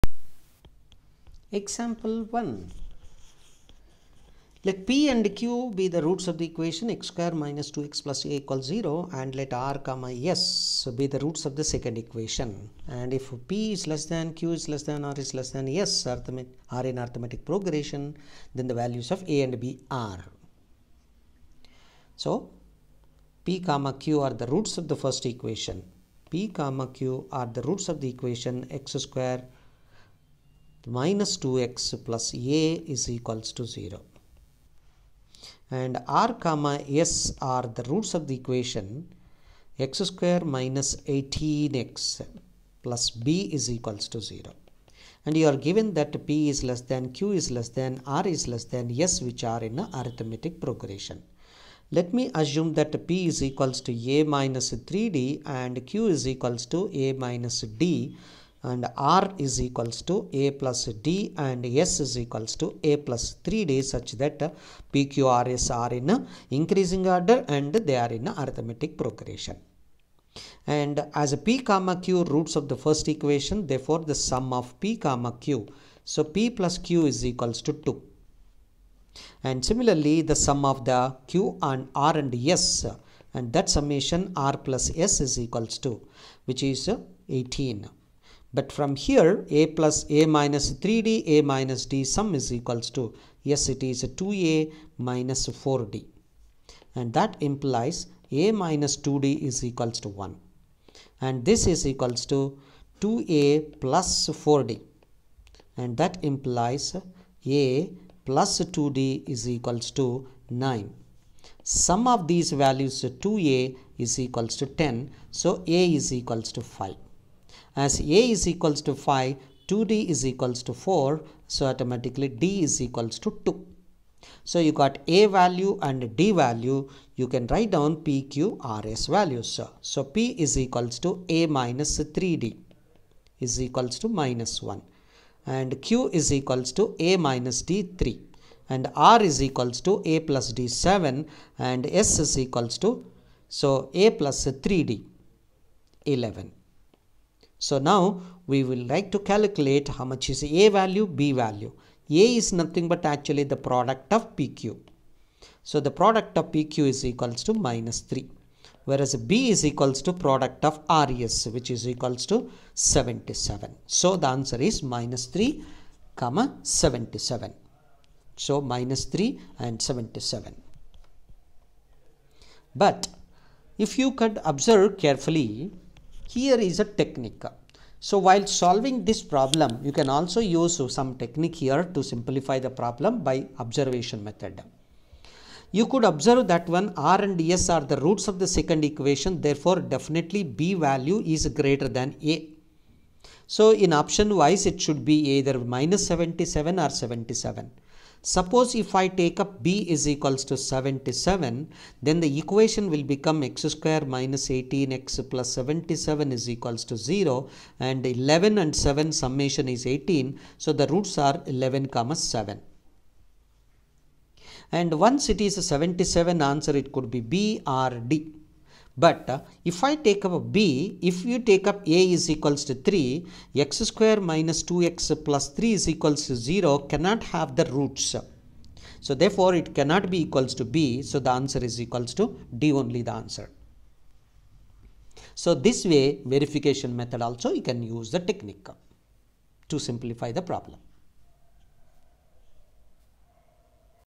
PU 1 | Stage 2 | Maths | Advanced Quadratic Equations | Lecture 3 | Slide 9 | EX 1
Welcome to Concept Videos for PU 1! This slide will explain Example 1 of Advanced Quadratic Equations. We are sure you will have an effective learning experience.